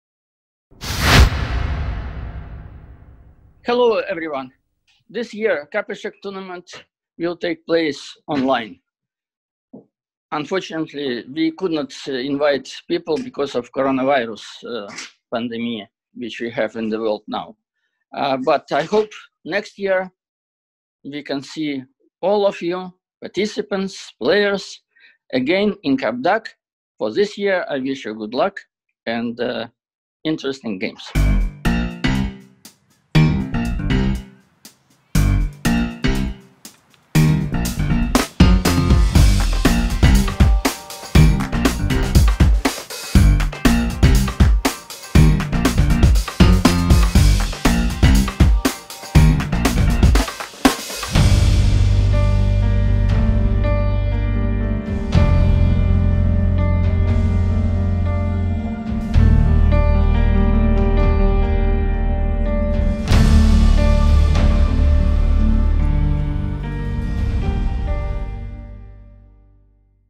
Trophée Capechecs Online 2020
Trophée Capechecs Online 2020 du 23 au 25 octobre, en ligne sur Europe-Echecs. Commenté par Andreea-Cristina Navrotescu (WIM) et Matthieu Cornette (GM)